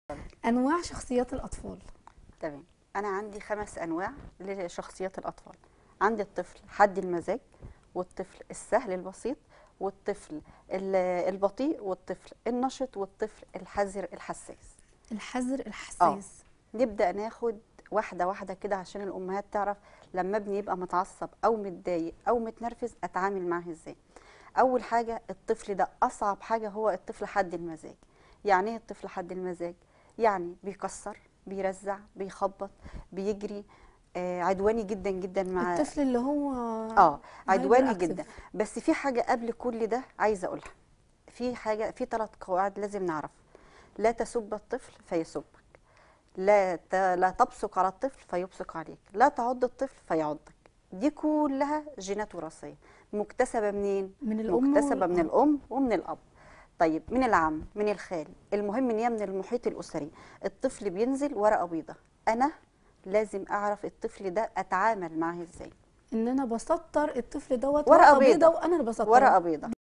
شكل تاني | تعرف علي أنواع شخصيات الأطفال وكيفية التعامل مع نوعية طفلك
تقديم | رانيا مصطفي
وفي ضيافتها | أ / محمد سعد
" مدرس تأسيس وتدريس اونلاين وصاحب مبادرة بدون تعقيد "
د / غالية محمد جويده
" استشاري اسري ودكتوراه في الصحة النفسية "